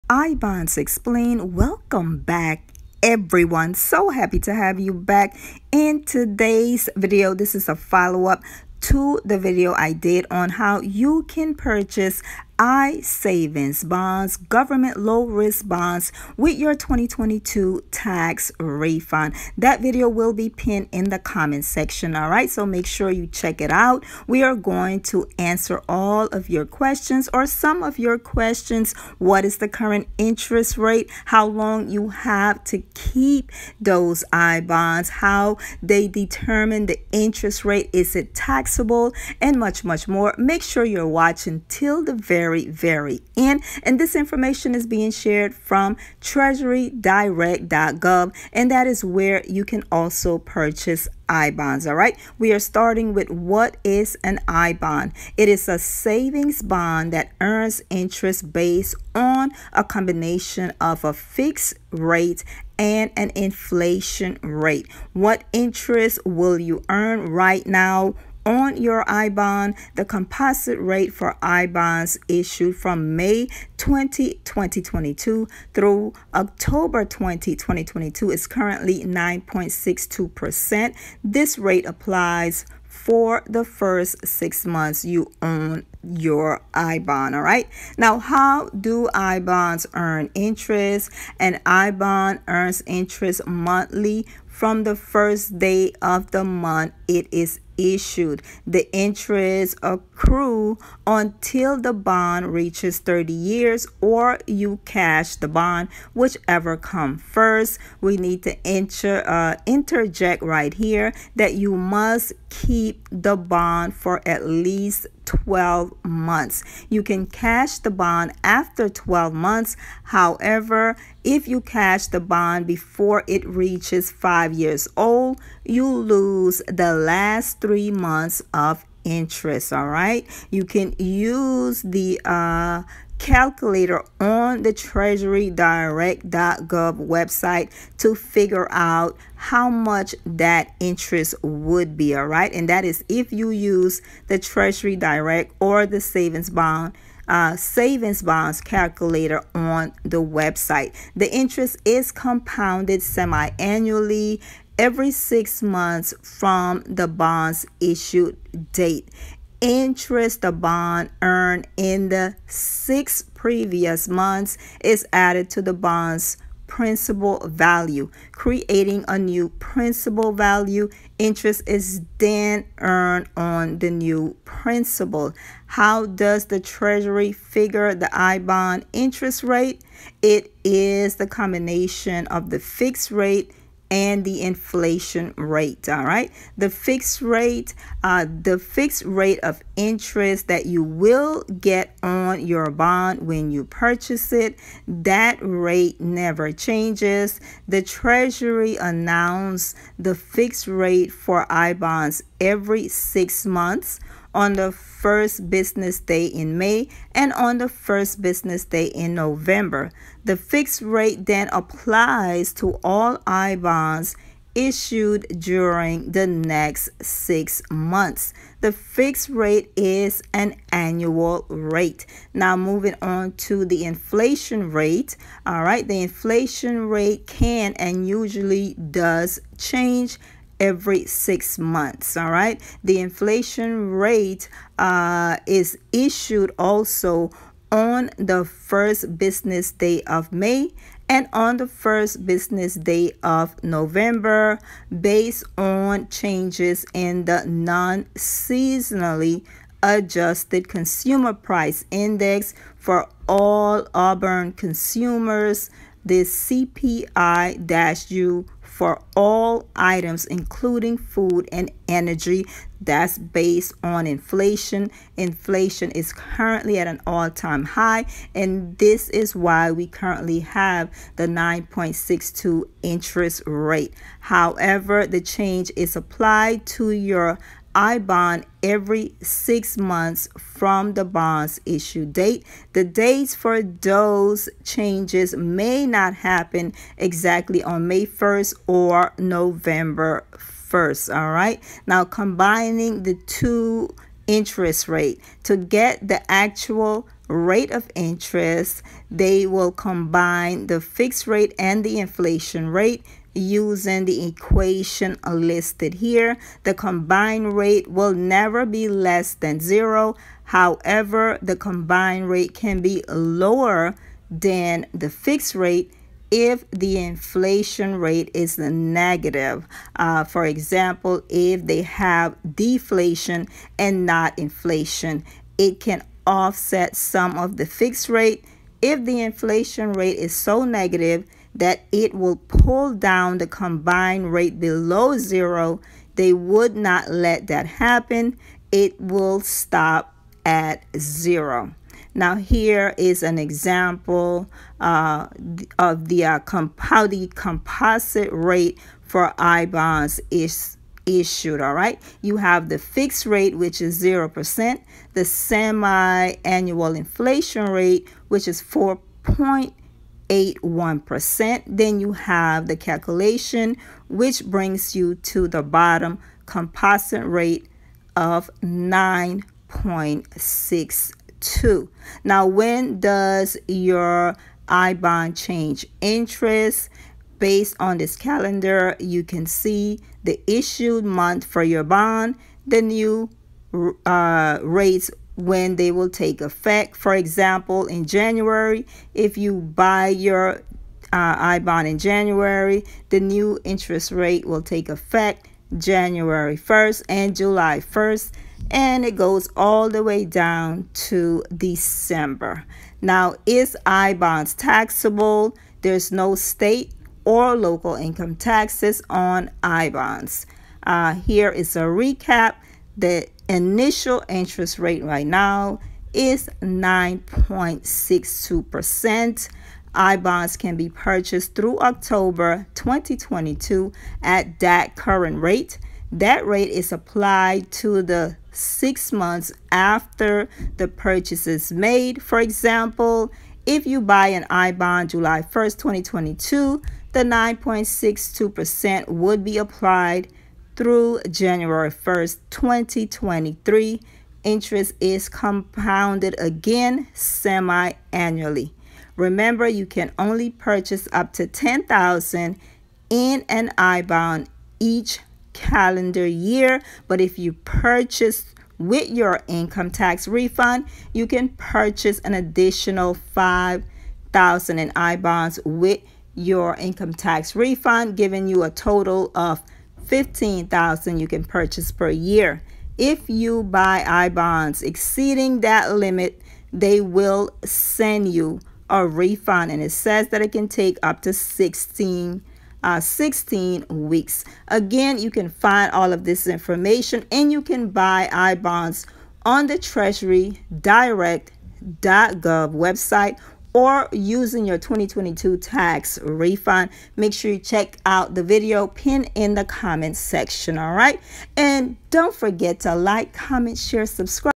I Bonds Explained
Explaining all of the key factors of i bonds, you need to know before purchasing i bonds.
This information is shared from the treasury direct dot gov website. I bonds interest rate
where to purchase i bonds,  how long you have to keep i bonds before you can cash in your
i bonds.

There’s a lot of interest in I bonds right now because for the next 6 months beginning May 2022, the interest rate will be 9.62% this is an investment with extremely low risk. In this video, I'm going to explain what I Bonds are